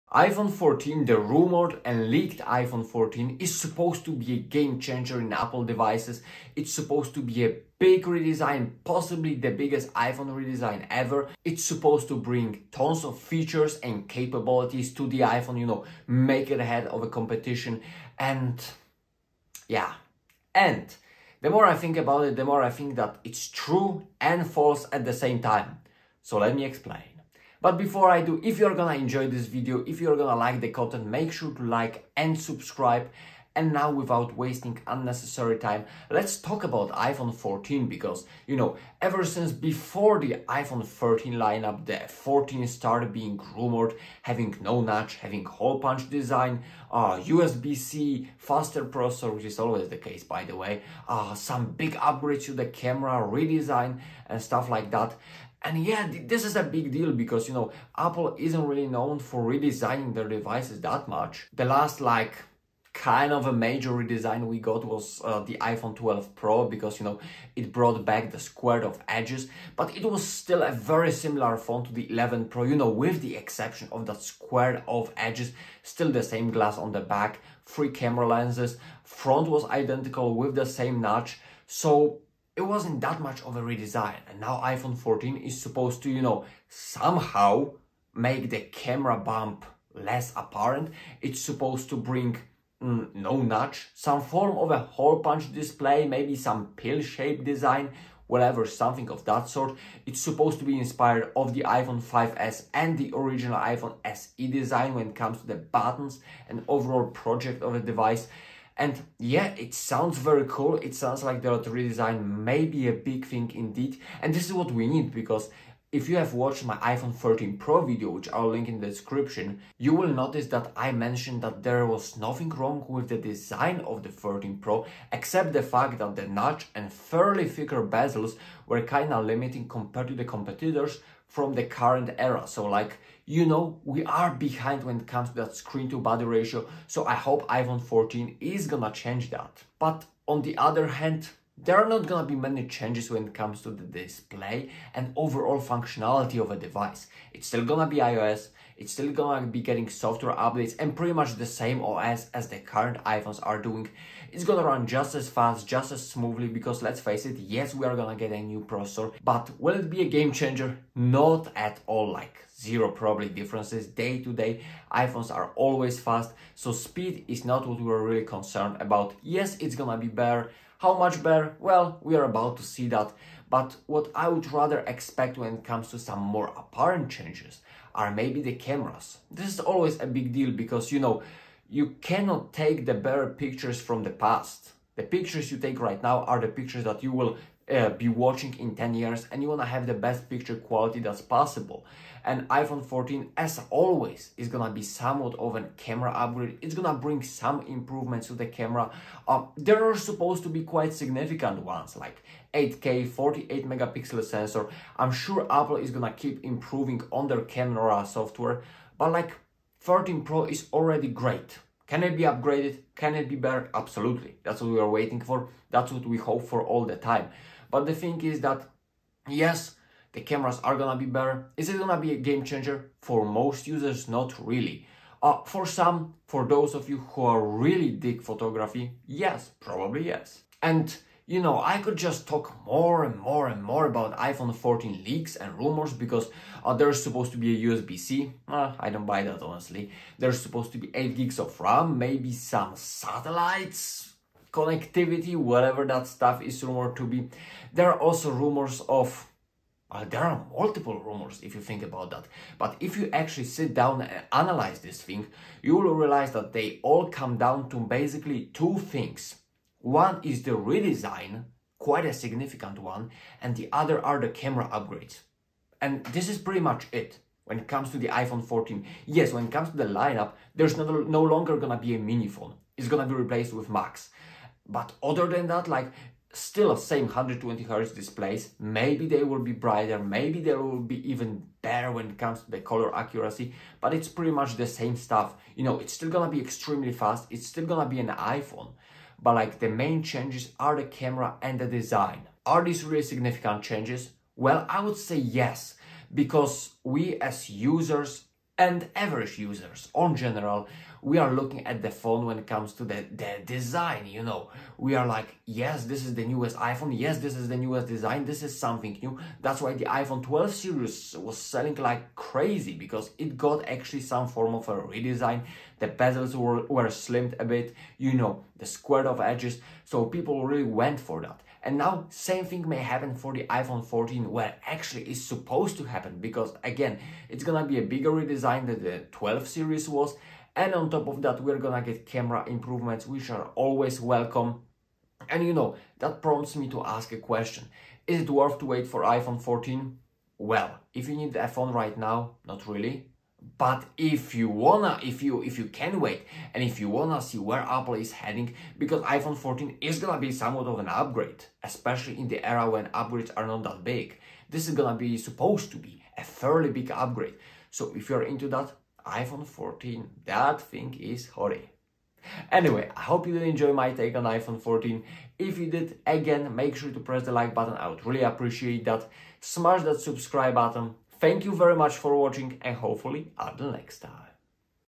iPhone 14 - ALL questions FINALLY answered! All You NEED to know!
iPhone 14 all you need to know! Latest iPhone 14 leaks and rumours! In this video we discuss what iPhone 14 is gonna change, if Apple iPhone 14 will be really as big of a redisign as people say, and we make sense of all the leaks and rumours of the upcoming iPhone 14. Let‘s talk iPhone 14!

00:00 Intro- iPhone 14 a game changer?
0:37 iPhone 14 - rumours and leaks
1:01 iPhone 14 redesign
2:20 iPhone 14 no real changes here?
2:55 Camera upgrades iPhone 14
3:50 iPhone 14 what really changes
4:50 is iPhone 14 a big upgrade really
5:39 worth waiting for iPhone 14